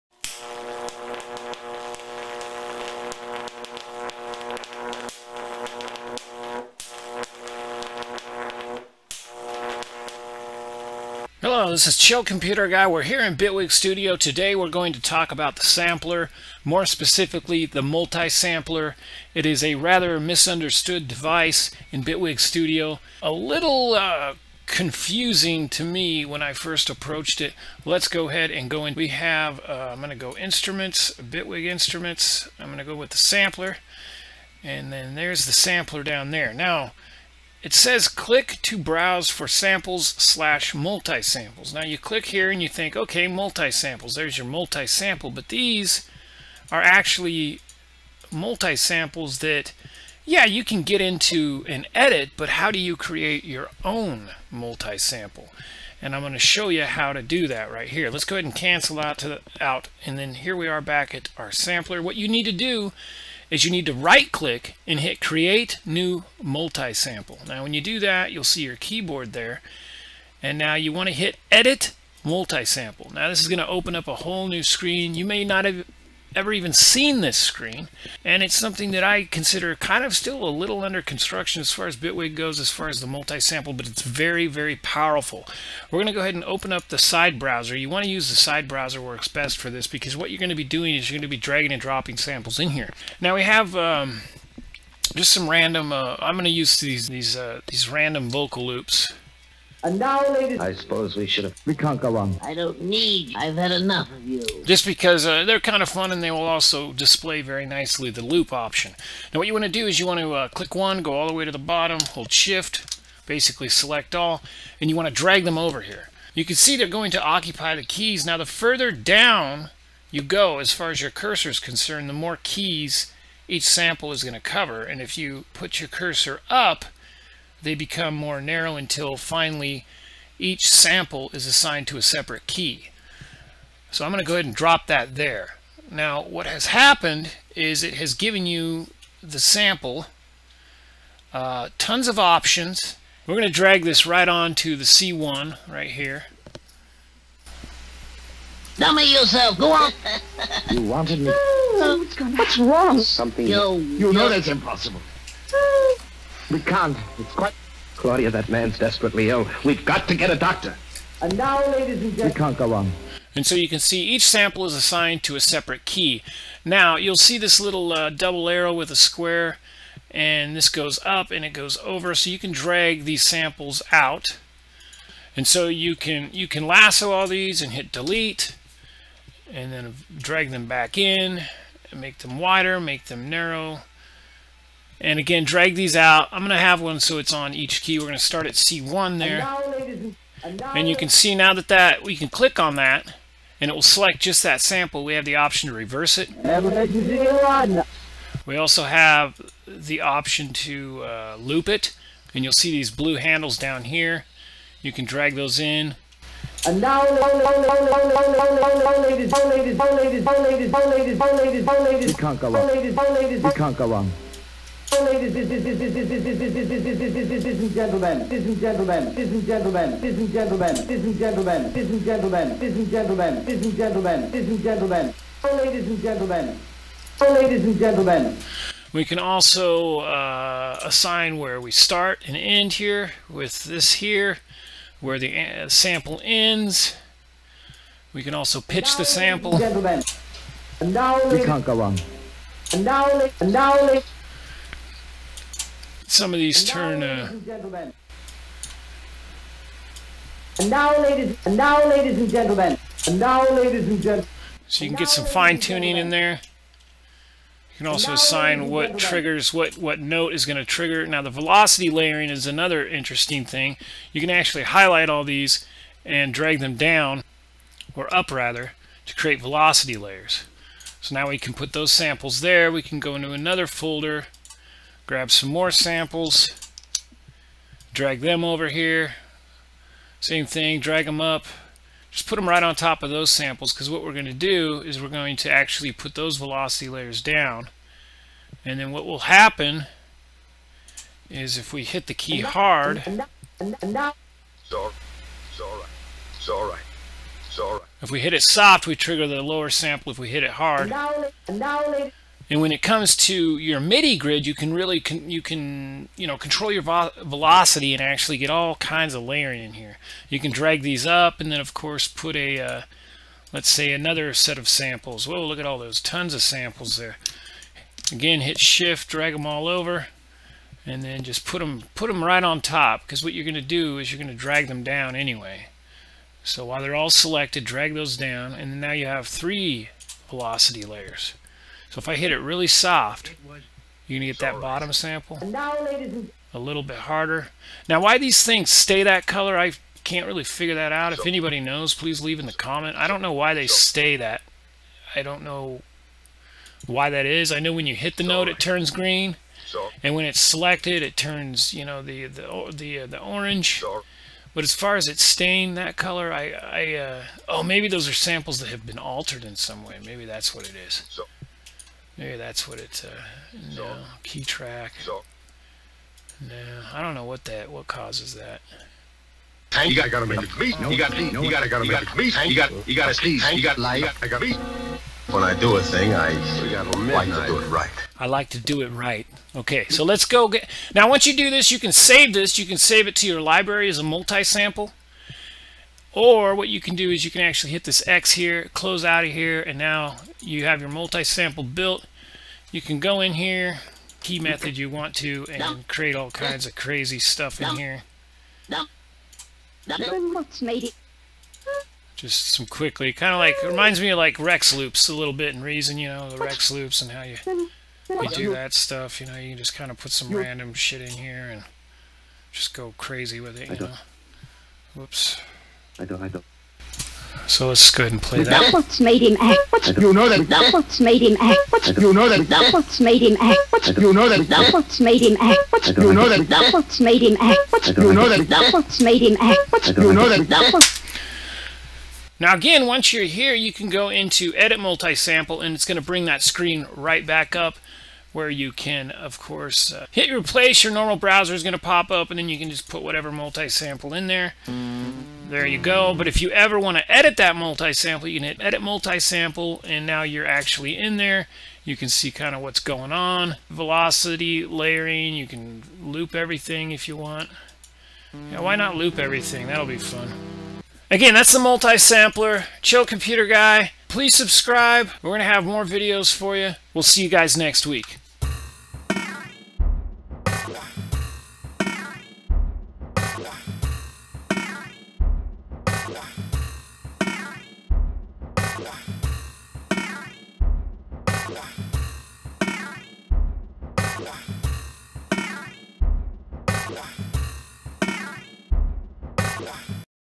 Bitwig Quick Tips: Using the Multi-sampler
This week in Bitwig Studio, We discussing the multi-sampler. The multi-sampler is a little confusing and in my opinion a work in progress, but so far it’s proven to be a very useful and powerful device for working with multiple samples. Combined with velocity layering and the ability to control each sample individually. This device is great for sound design and music creation.

Below is a special link to my other YouTube channel.